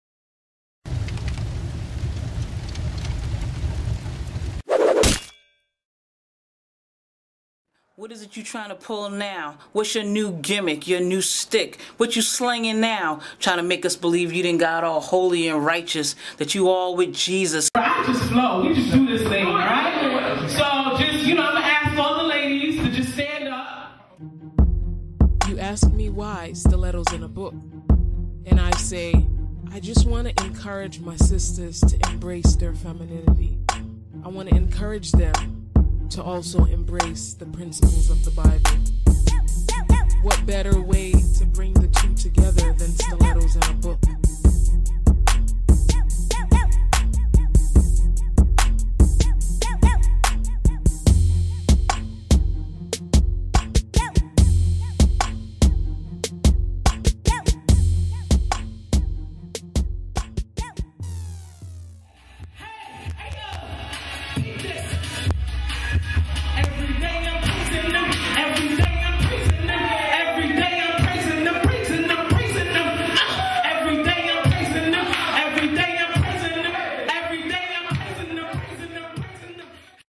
Serious - Official SAAB Promo Video
Serious, envisions a tour that will encourage women to embrace their femininity and embrace the principles of the Bible. Without sounding too "religious" she has decided to dub the tour Stilettos & A Book. Stilettos because it conjures up thoughts of a woman looking her best and Book because women need the Book for their daily survival.